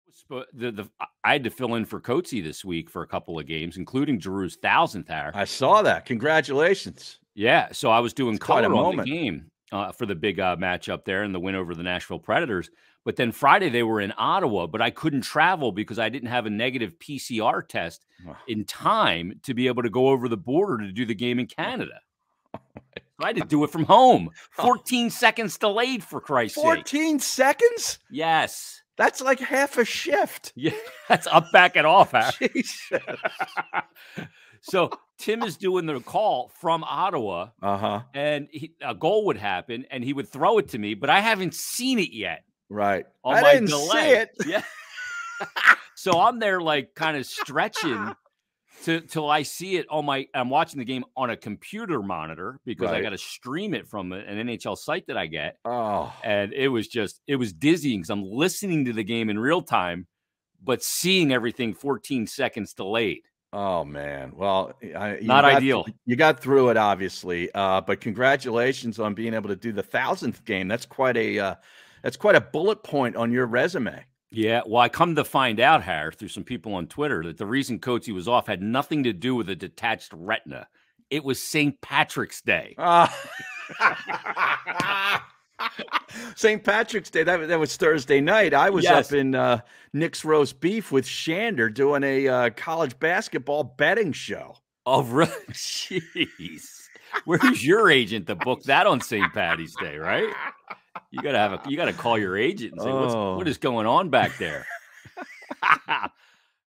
JASON MYRTETUS DOES FLYERS RADIO
In episode 16 of "Ah Jeez" with @harrymayesTU and @jasonmyrt, the guys talk about Jason filling in as color commentator for the Flyers on the radio.

Jason had to do a game from his house while the team was in Ottawa on Friday. It was 14 second delay. He also got to do Giroux's 1,000th game!